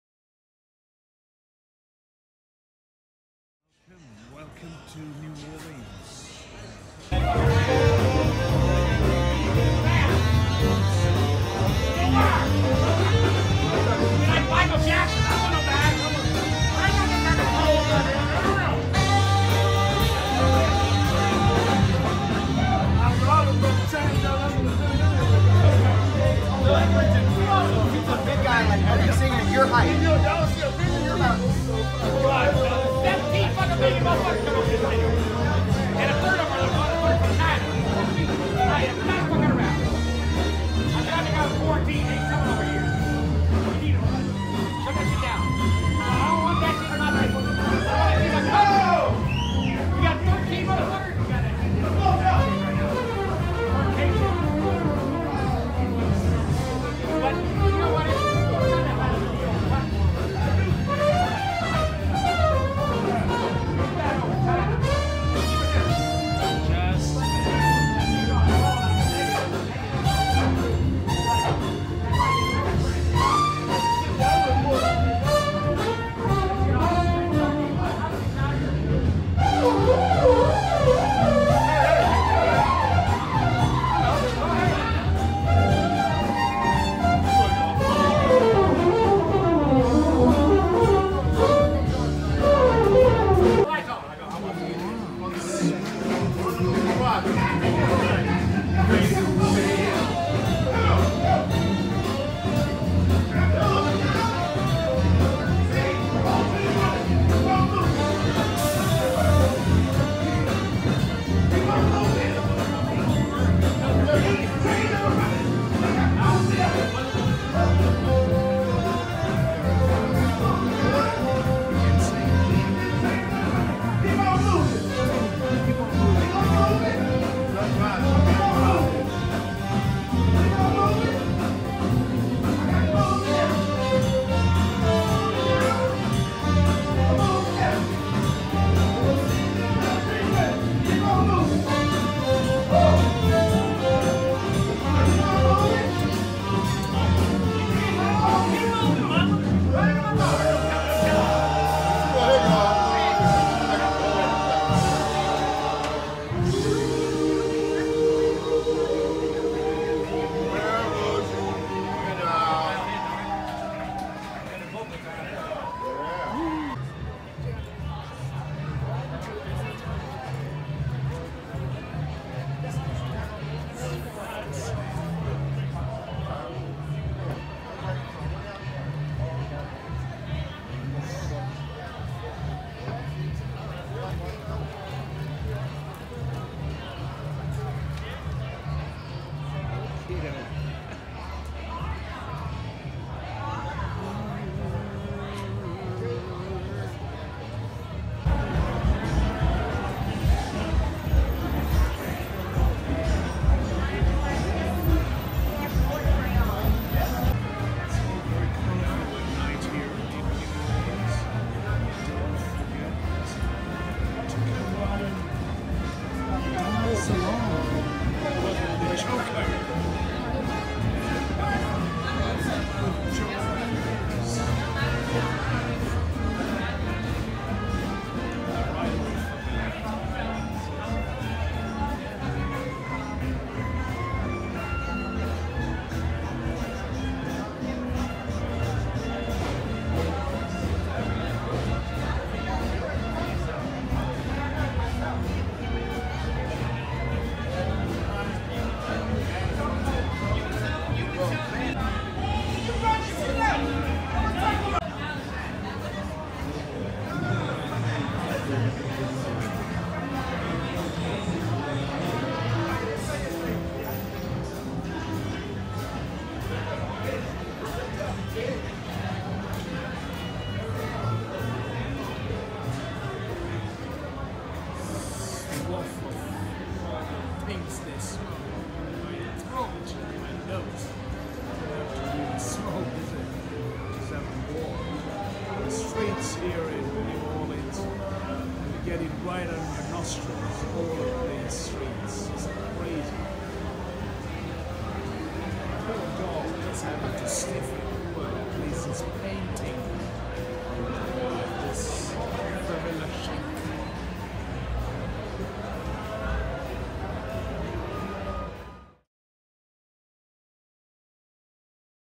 Jazz Clubs and Bars of New Orleans
New Orleans is the best place in the world to hear live music. The birthplace of jazz, this is the town that also birthed jazz greats like Louis Armstrong.